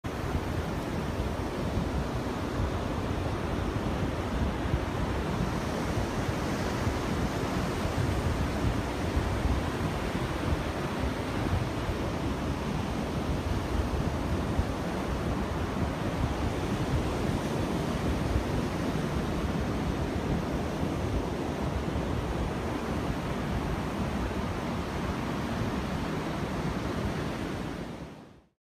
Pranayama Practice ✺ Memory from Fuerteventura
Preparation for a seated pranayama practice in a unique place in Fuerteventura.